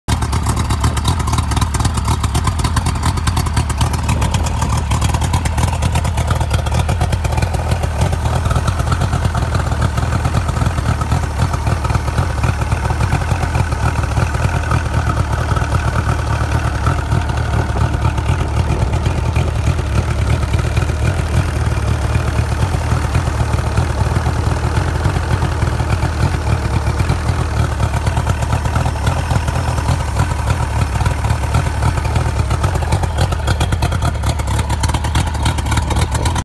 THT: Water Jacket Cover Replaced-No Leaks!
Just a thank you to my brothers at The Hull Truth, you gave me enough information and motivation to get this repair done!

Still needs a tune up but NO LEAKS.